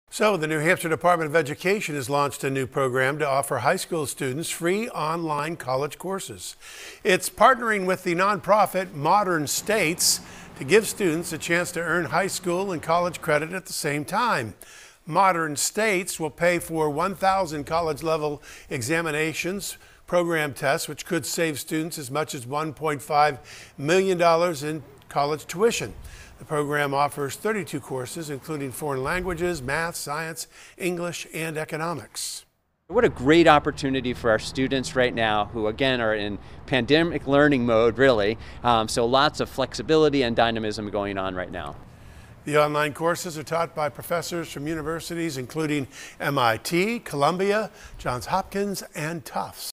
New program offering free college classes to Granite State High Schoolers
The New Hampshire Department of Education launched a new program to offer High School students free online college courses.